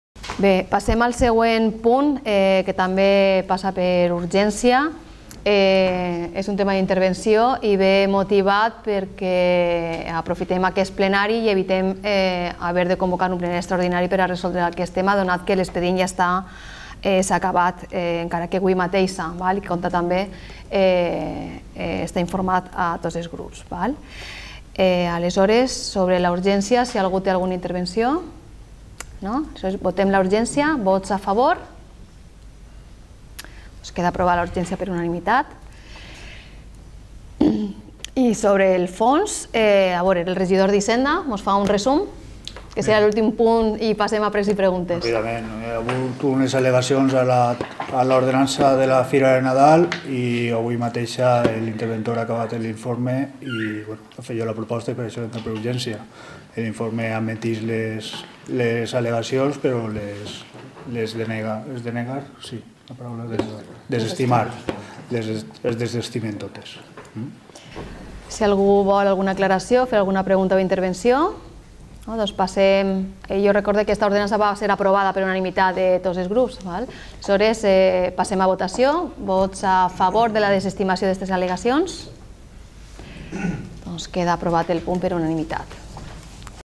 AL·LEGACIONS/RECLAMACIONS ORDENANÇA FIRA NADAL - Ple Novembre Xixona 2016
20. Proposta de desestimació de les al·legacions/reclamacions interposades per sra. I. A. C. R. Enfront de l'acord provisional d'imposició de l'ordenança fiscal reguladora de la taxa per prestació de servicis per a la realització de la Fira de Nadal de Xixona.

Punt corresponent a l'ordre del dia de la sessió ordinària del Ple de la Corporació que va tindre lloc el 24 de novembre de 2016.